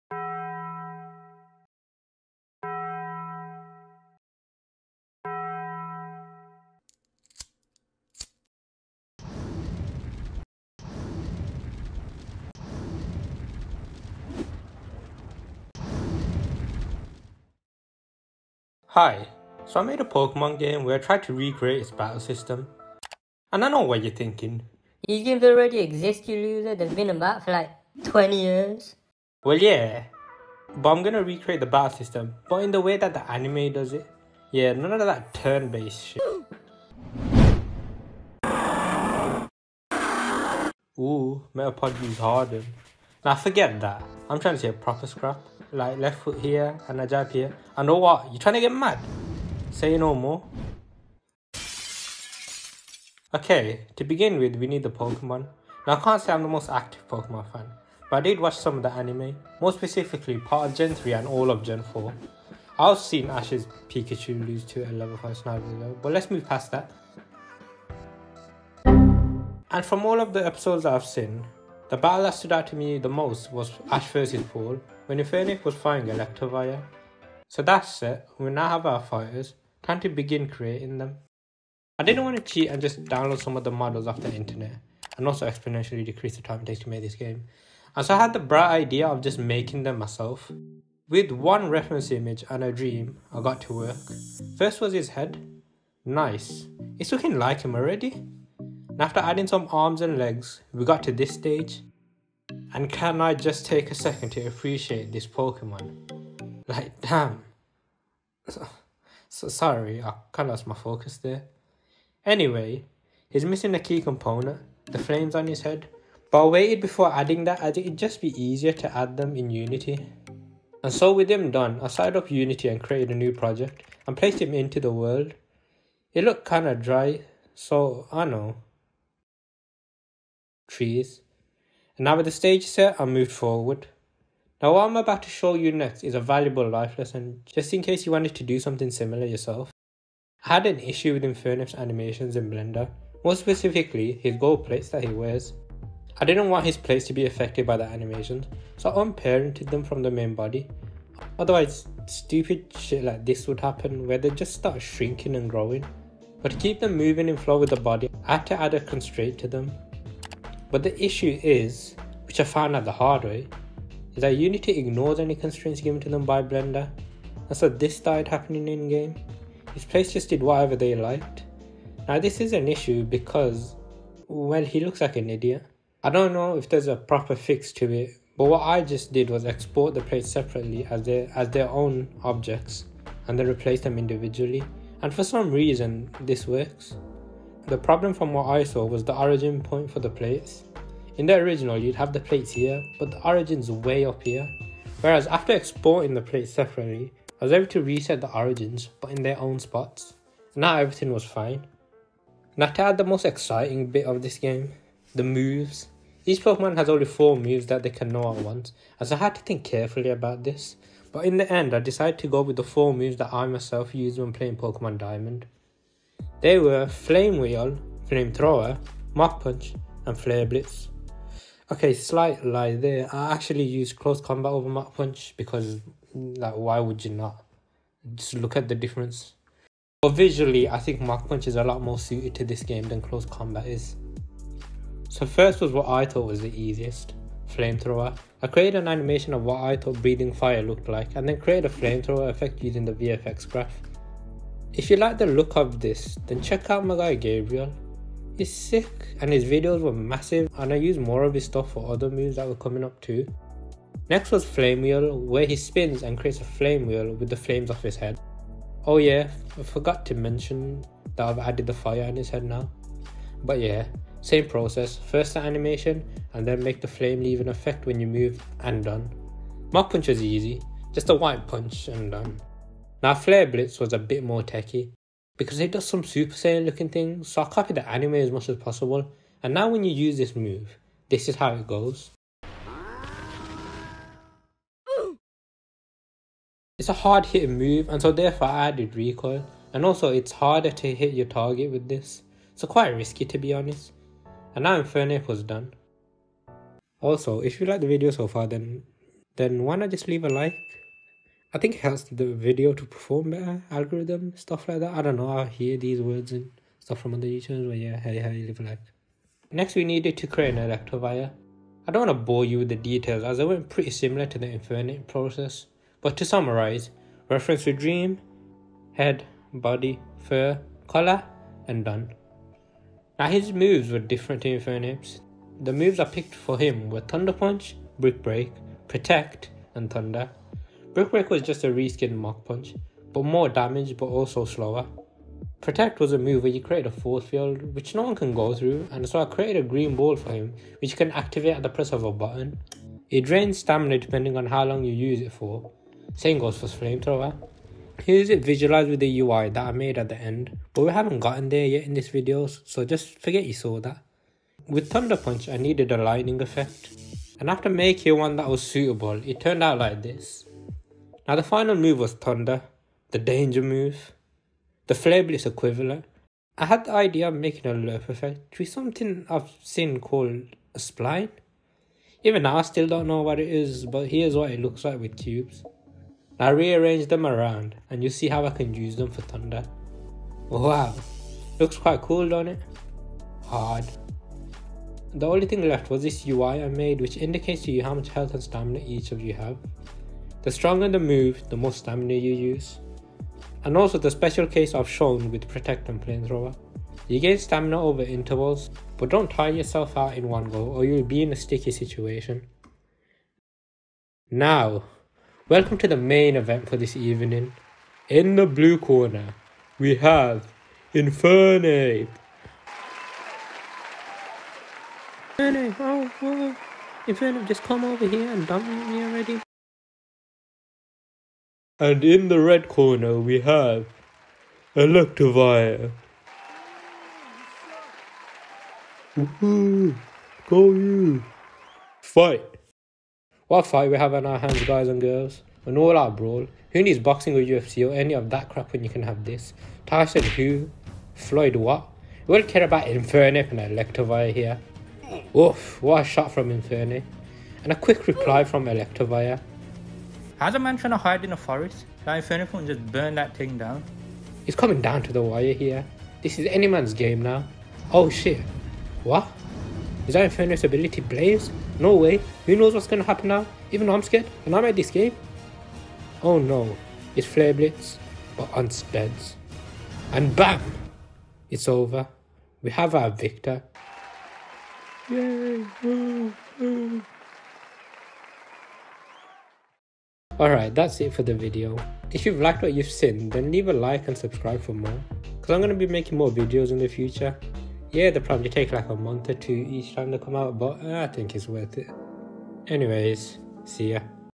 Recreating Pokemon, But in Anime Style
In this video, I remake the battle system from the Pokemon games, but in the way that the anime does it.